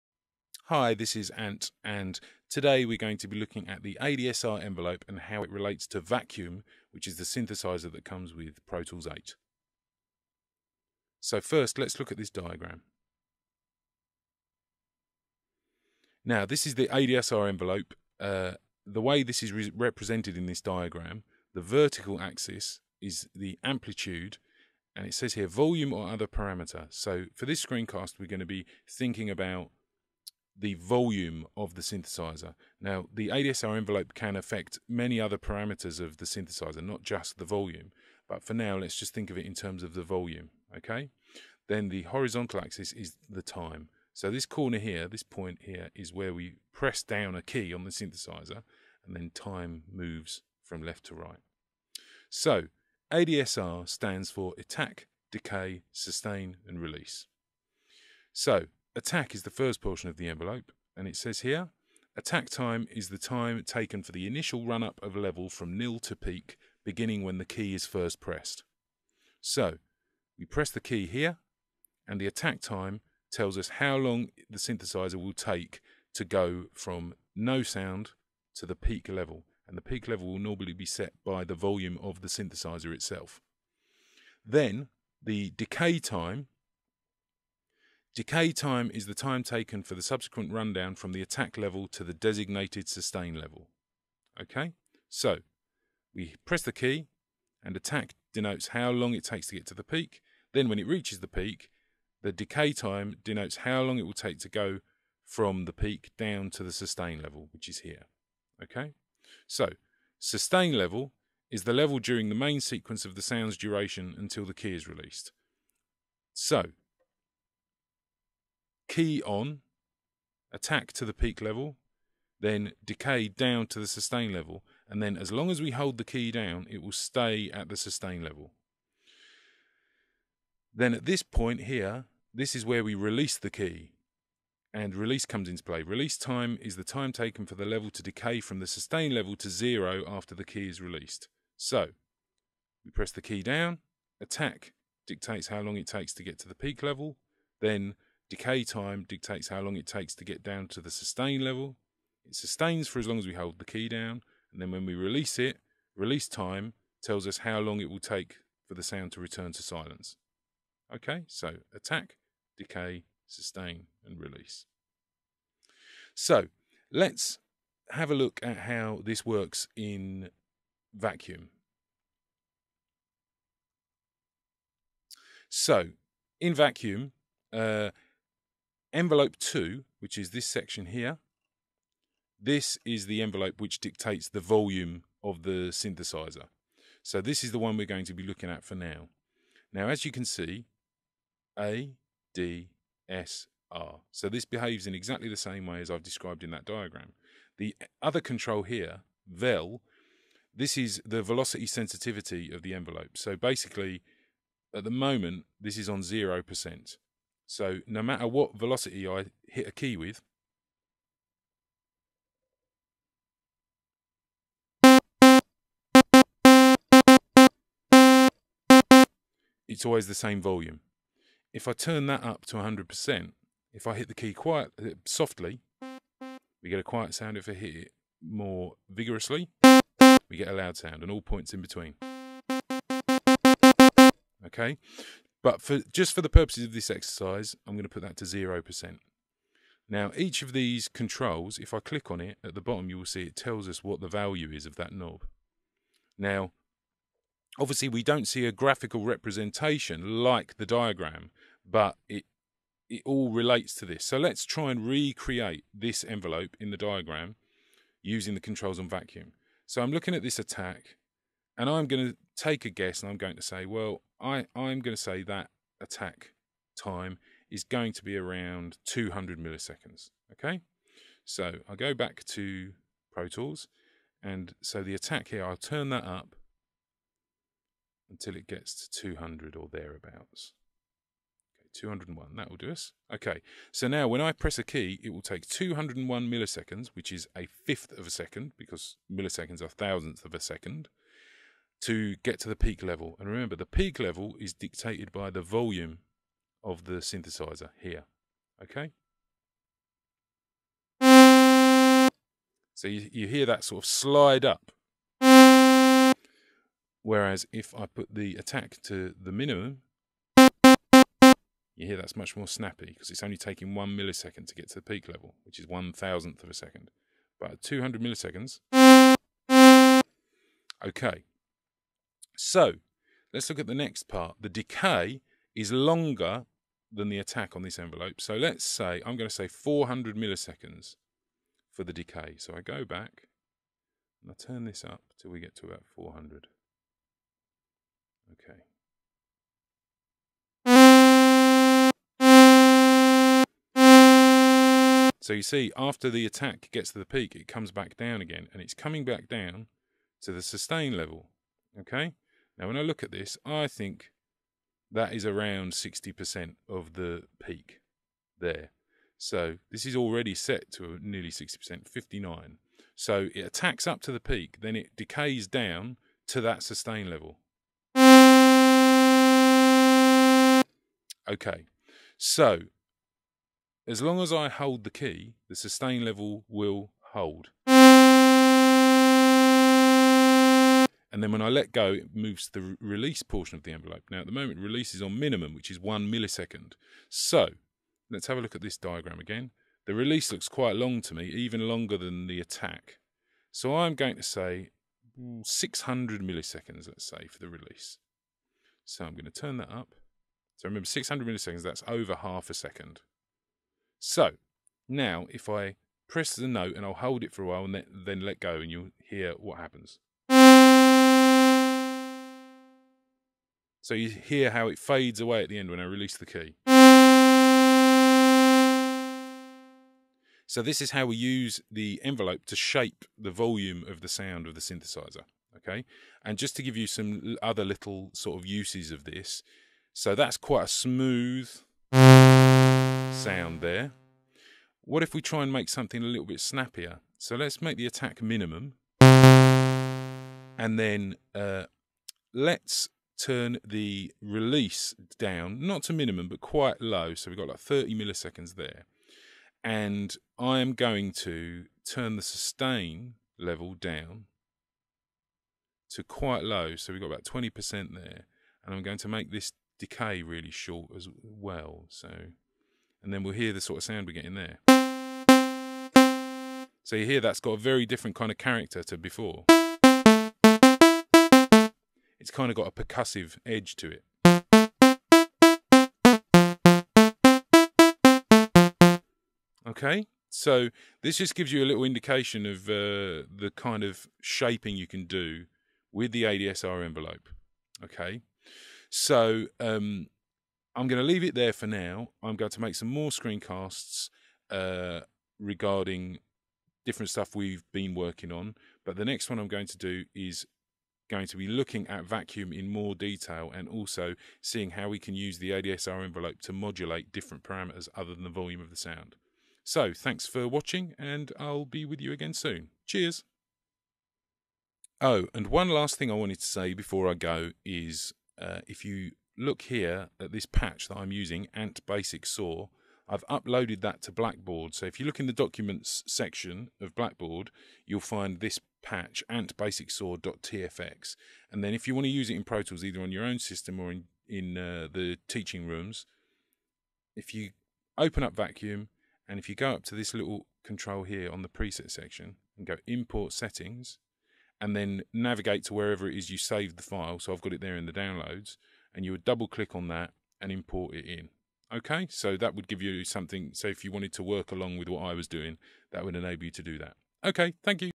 AU405 - ADSR envelopes & Vacuum
This screencast serves as an introduction to ADSR envelopes, and how they function within Vacuum, the synthesiser plugin that is included with Pro Tools 8